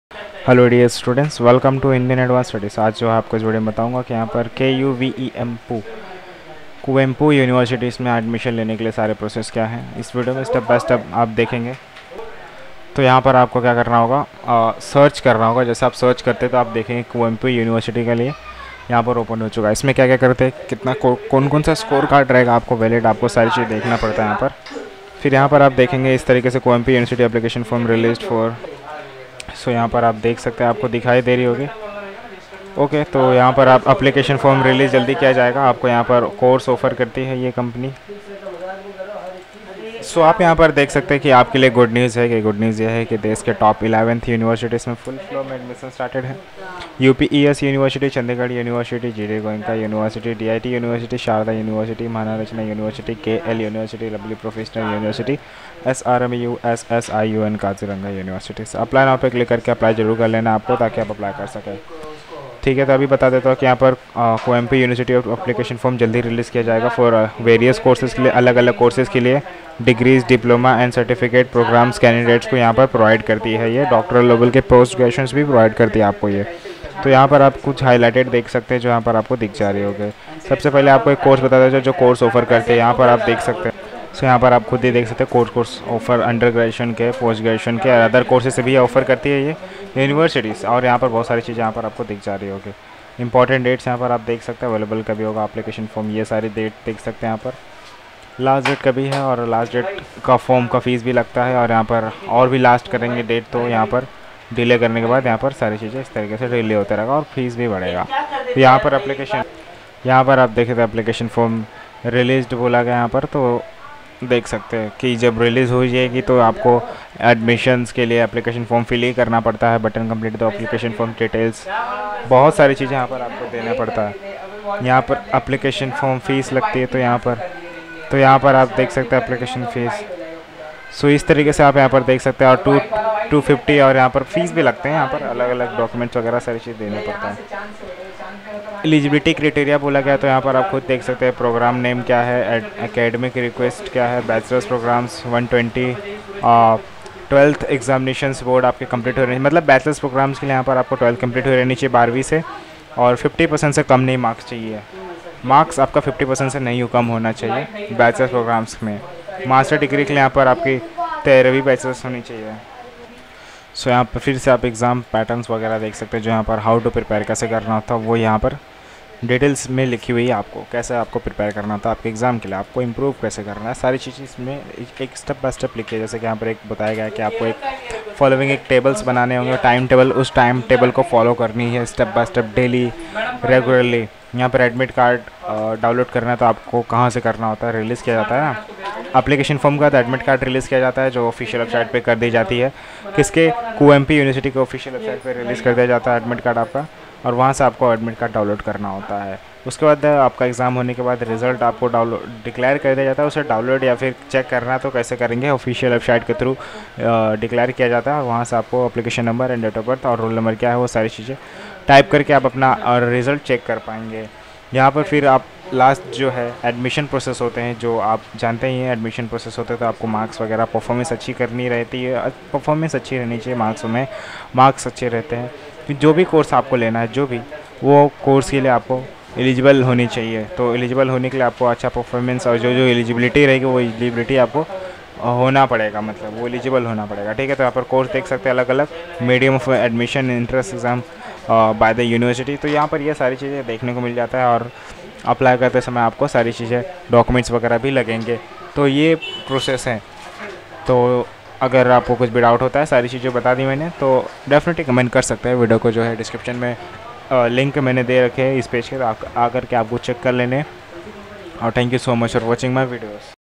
Kuvempu University Admission 2021 : Application ,Exam Pattern, Eligibility Criteria, Exam Syllabus
Kuvempu University 2021 Application Form will be Releasing soon for Various Courses.